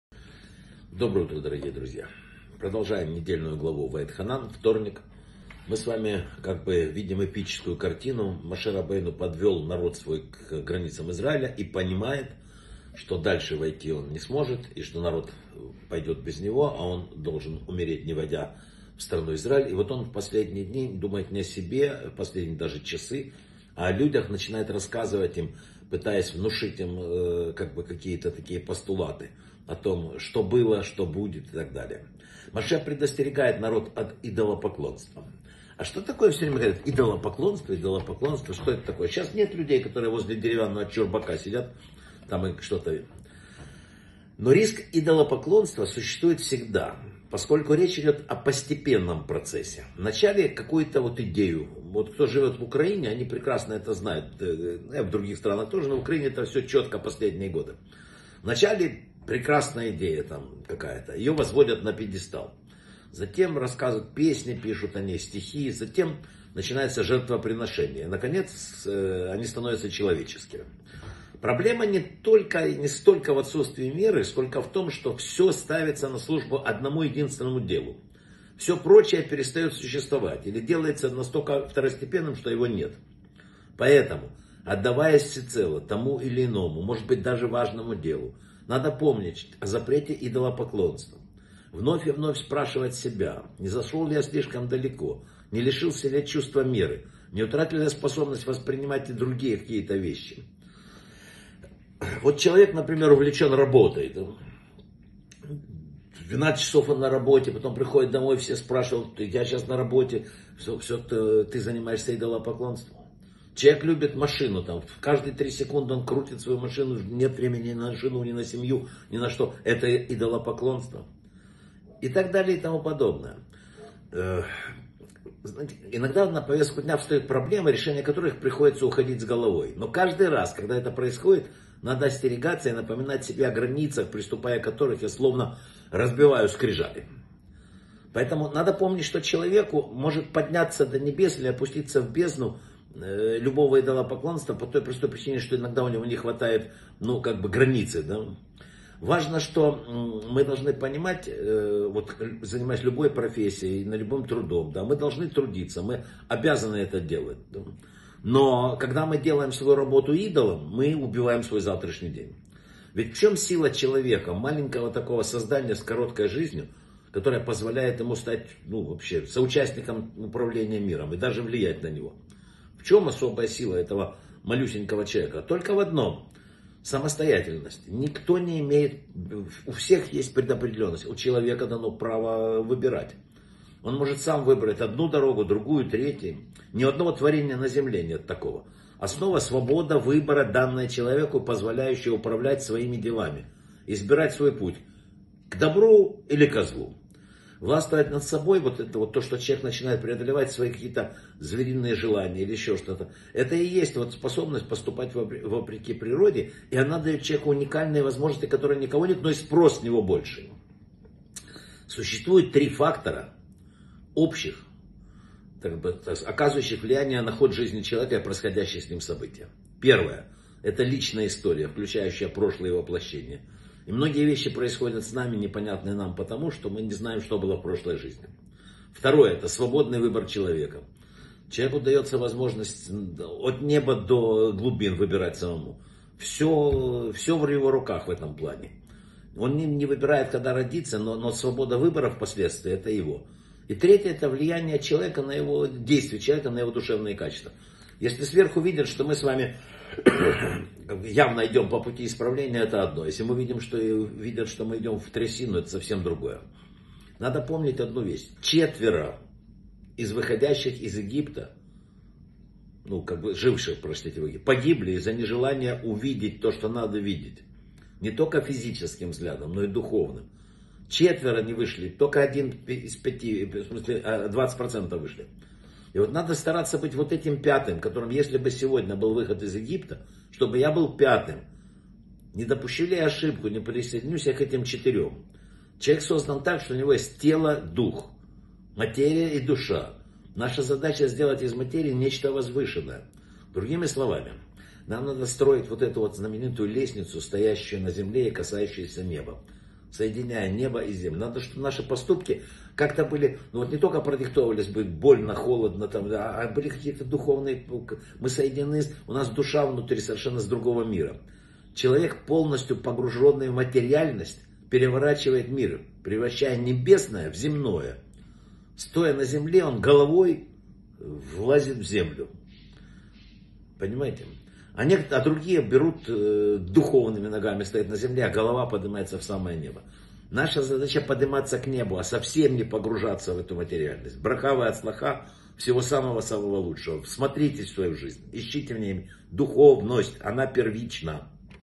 📘 Идолопоклонство - коварный враг на все времена. Недельная глава Ваэтханан | Вадим Рабинович
В недельной главе Моше рабейну предостерегает евреев от идолопоклонства. А в чем суть этого понятия сегодня?
Проблема не только в отсутствии веры - какую еще ошибку часто допускают люди?
О чем надо помнить, всецело отдаваясь даже полезному и важному делу?
Важность соблюдения границ.
В чем большая сила маленького человека, позволяющая влиять на мир?
Три фактора, оказывающие влияние на течение жизни человека.
Каким стремлением должны быть продиктованы наши действия?